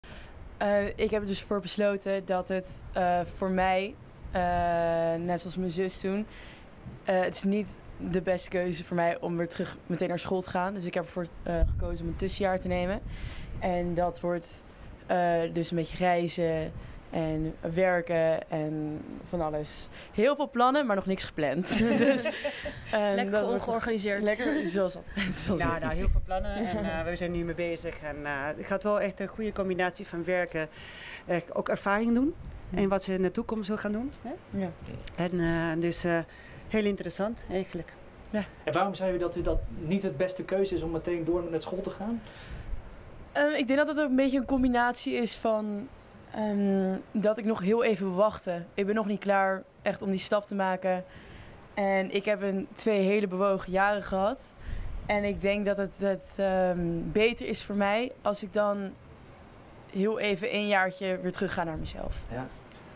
Dit gaat prinses Alexia na de zomer doen
Prinses Alexia is net terug uit Wales. Wat gaat ze het komende jaar doen? Studeren? Reizen? Ze vertelt het zelf op het strand in Den Haag.